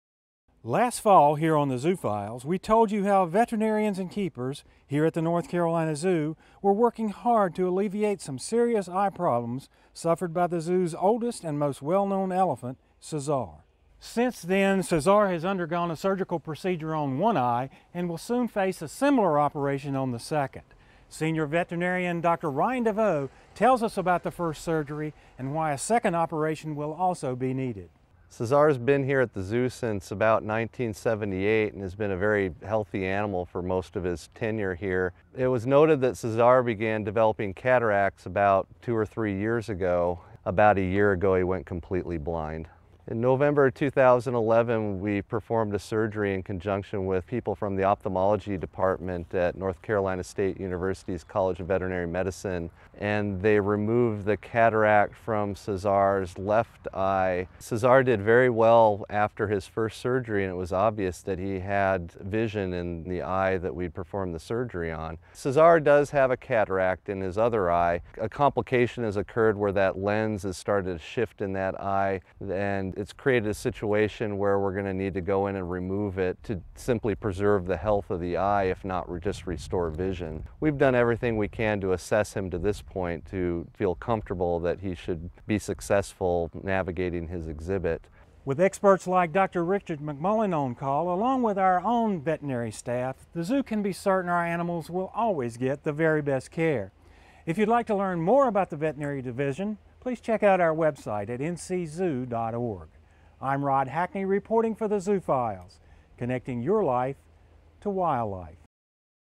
Elephant Eye Surgery
C'sar, the North Carolina Zoo's oldest and most well known elephant was blinded by cataracts until a surgical procedure restored his sight in one eye. The procedure was so successful that C'sar can now navigate his exhibit once more. A second surgery is scheduled on his other eye